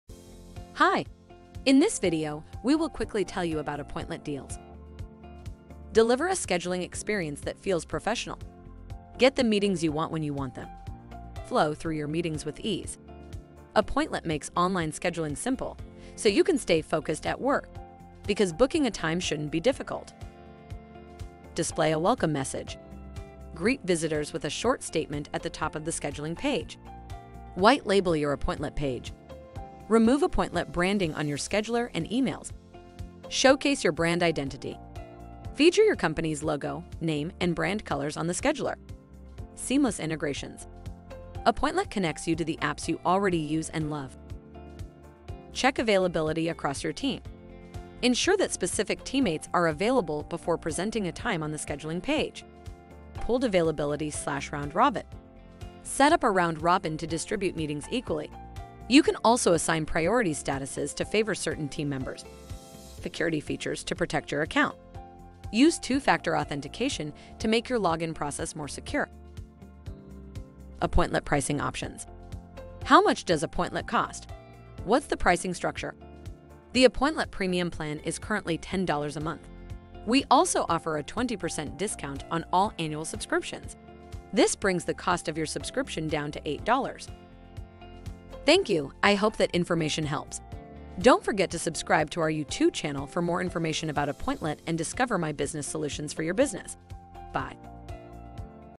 Appointlet Deals
Hi! In this video, we will quickly tell you about Appointlet Deals

Deliver a scheduling experience that feels professional
Get the meetings you want when you want them

Flow through your meetings with ease
Appointlet makes online scheduling simple, so you can stay focused at work. Because booking a time shouldn’t be difficult.

Display a welcome message
Greet visitors with a short statement at the top of the scheduling page

White label your Appointlet page
Remove Appointlet branding on your scheduler and emails

Showcase your brand identity
Feature your company’s logo, name, and brand colors on the scheduler

Seamless integrations
Appointlet connects you to the apps you already use and love

Check availability across your team
Ensure that specific teammates are available before presenting a time on the scheduling page

Pooled availability / Round robin
Set up a round robin to distribute meetings equally. You can also assign priority statuses to favor certain team members

Security features to protect your account
Use two-factor authentication to make your login process more secure

Appointlet Pricing Options
How much does Appointlet cost? What’s the pricing structure?
The Appointlet premium plan is currently 10 dollars a month. This brings the cost of your subscription down to 8 dollars.

00:00 - 00:04 - Welcome to our video!
00:05 - 01:09 - Appointlet Deals & Features
01:10 - 01:27 - Appointlet Pricing Options
01:28 - 01:38 - Thank you!

USEFUL RESOURCES